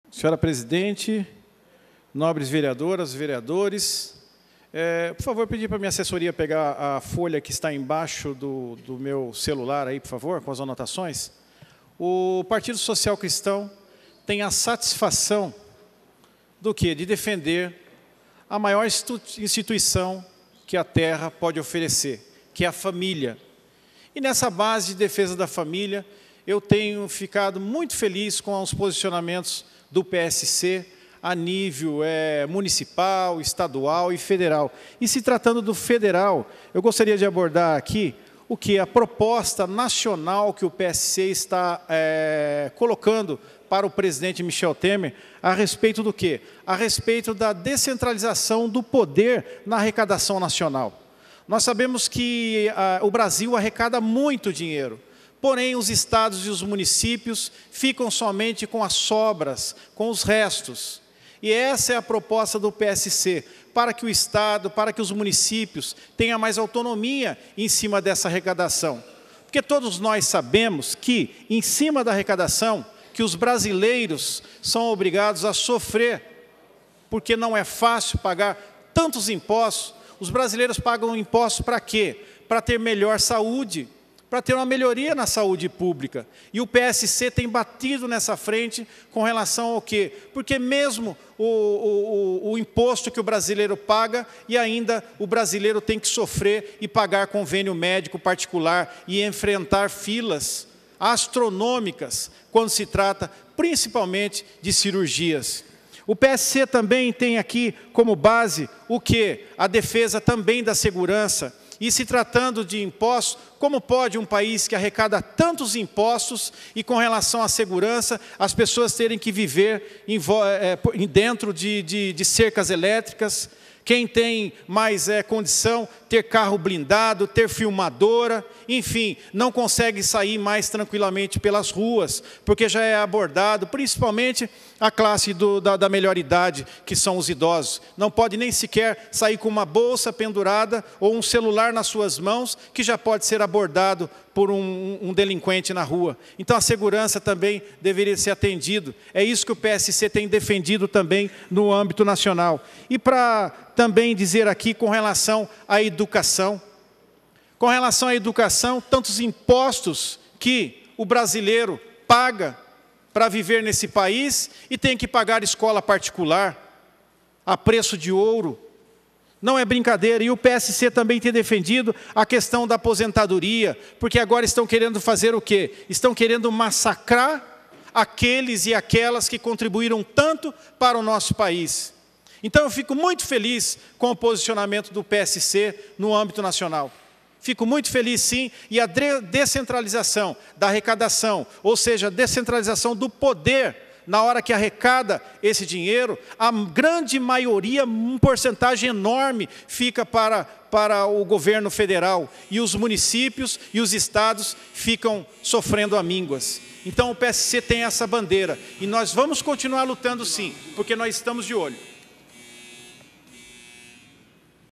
Horário de Liderança – Vereador Fernando da Ótica Original (PSC) 22/2/2017
Horário de Liderança – 4ª Sessão Ordinária, realizada no Plenário da Câmara de Jacareí, no dia 22 de fevereiro de 2017.